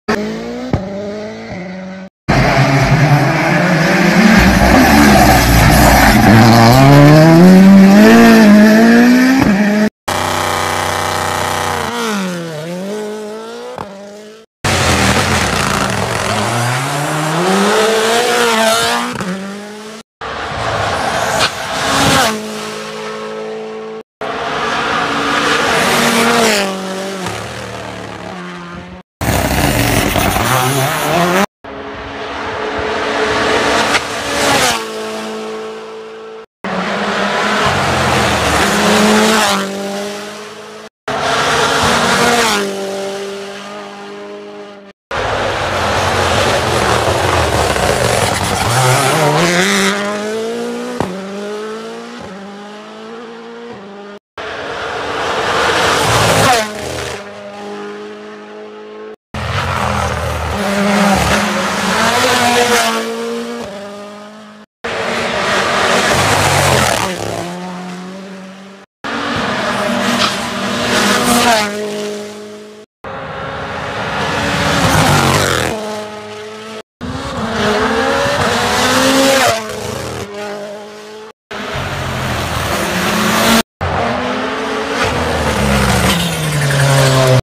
rally Montecarlo remix movi 2023
le bellissime immagini sui social video rielaborato per riviverli in sieme,test vetture